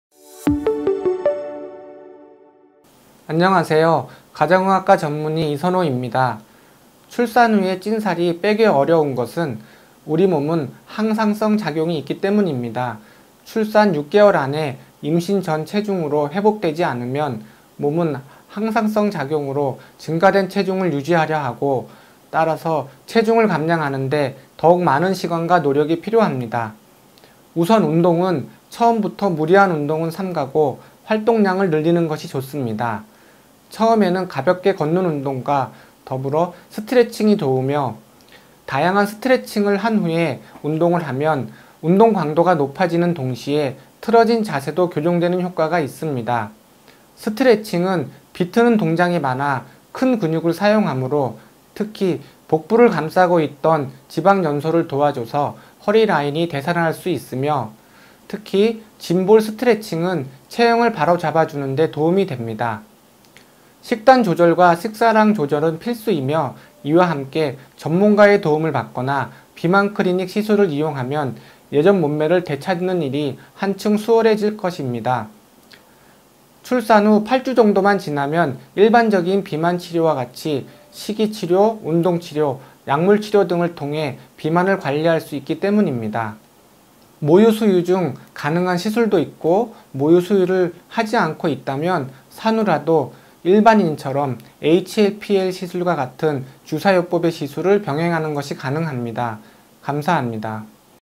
지방흡입 Q&A : 28. 산후다이어트, 출산 후 다이어트가 너무 어려워요_365mc병원
출산 후 망가진 몸매, 어떡하죠?
의사쌤이 알려주는 건강한 다이어트 방법!

출산 후 늘어난 살들과 빠지지 않는 살들!
몸매회복을 위한 운동과 치료법을
자세하게 알려드립니다!